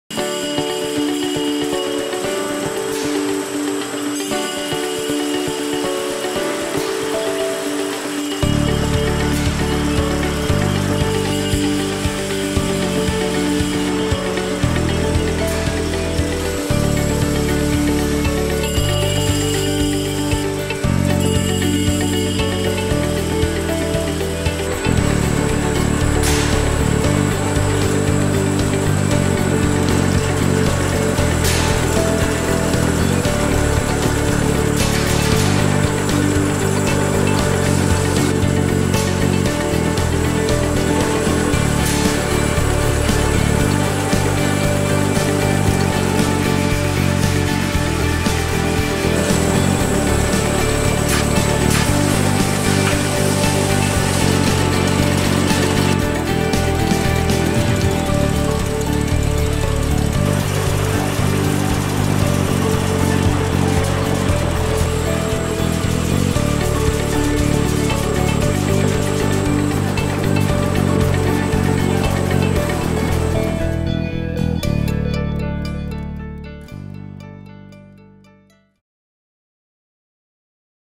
Elmar Piston Filler 54 Filling Stations with Dual Hook Up for Metal and Glass Containers
Elmar Rotary Valve Piston Filler 54 Filling Stations for Milk-Based Drinks with Dual-Hook Up for Metal and Glass Containers